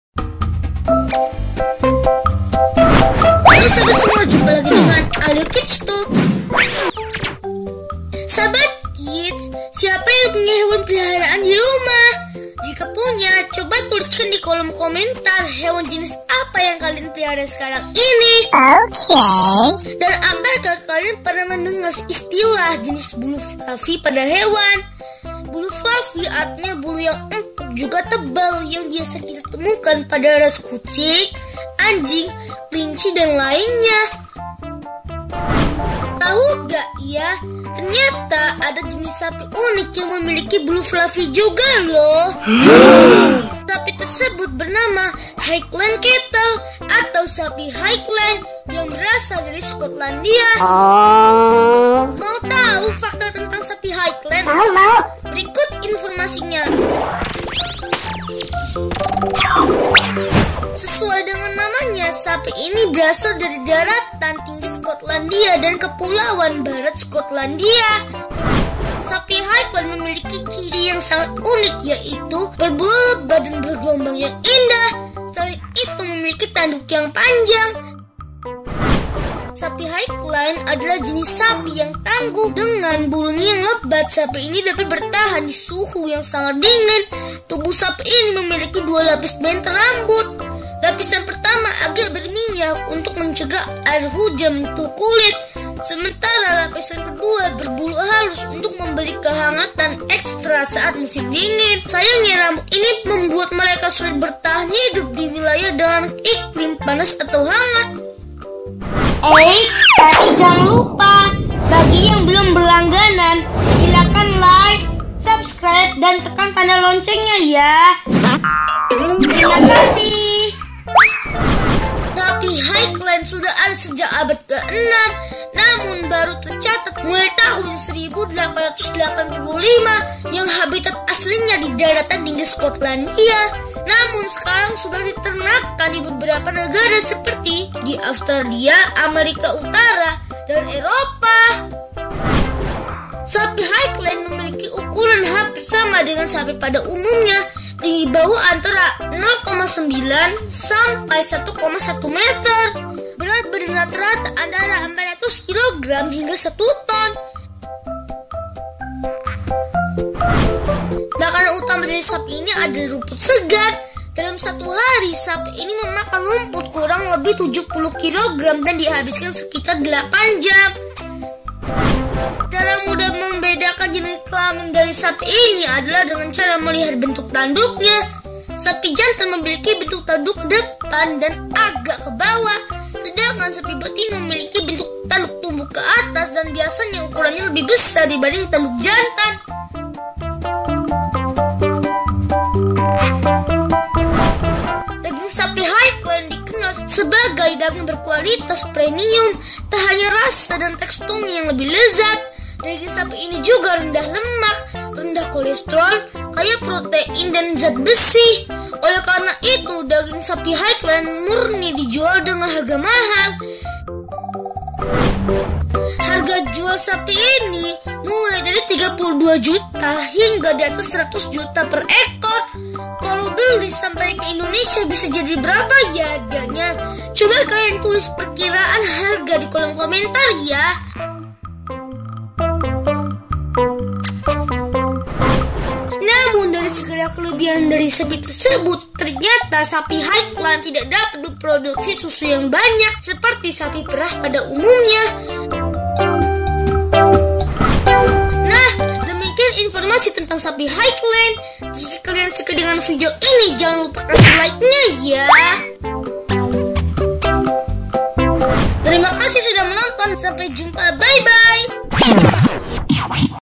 HIGHLAND CATTLE SPECTACULAR COW - SOFT FLUFFY - THE PRICE IS FANTASTIC
Hello Kids friends, see you again on the Alyo Kids Tube channel
In episode 06 " Tau Gak ya " will discuss about the type of COW which has an unusual shape, which has fur like the Angora cat
The name of the animal is "HIGHLAND CATTLE"
Let's look at the facts about the HIGHLAND CATTLE in this video...